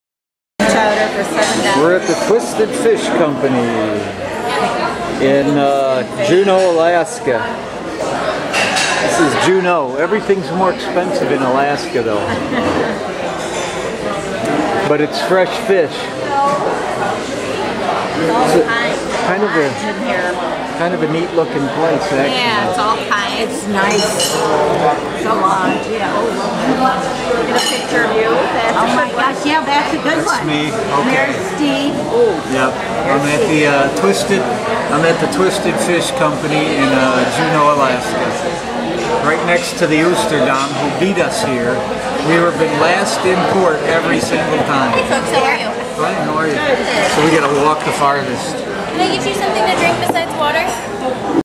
TWISTED FISH CO JUNEAU ALASKA
One of many vids of my August 2011 vacation.  We all had fried cod and fries.  The fish was fresh but a little overcooked.  Not cheap, but good food and good atmosphere.  I had the best amber O'doul's I've ever had.  Went good with the fish.